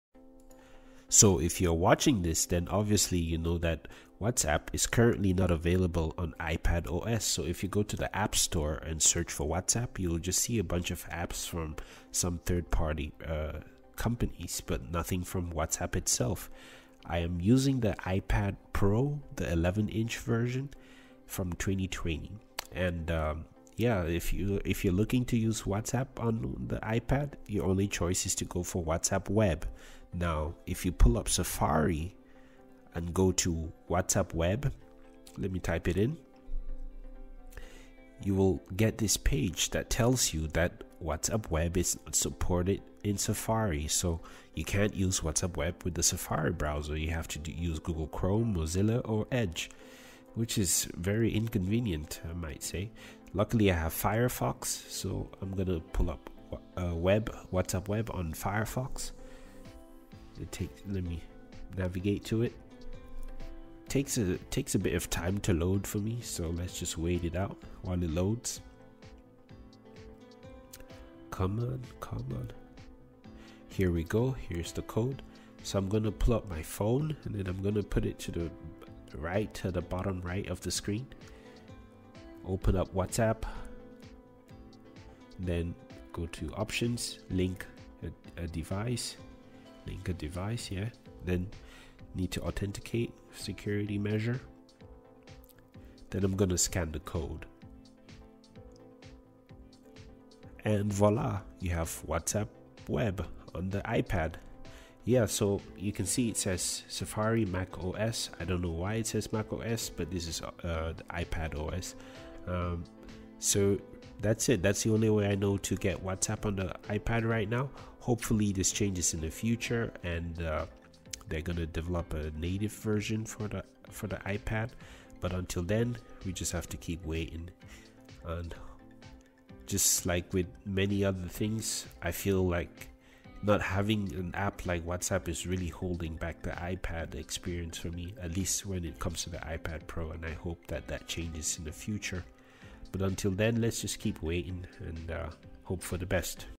WhatsApp (Web) on the iPad Pro
WhatsApp is currently not available on iPad OS, the alternative is to use WhatsApp Web. However, WhatsApp Web does not work on the version of the Safari Browser that is on the iPad. This means that one has to use a third-party browser to access WhatsApp Web on the iPad. While waiting for a native version of WhatsApp to be delivered for iPad OS, this is the solution that I have resorted to.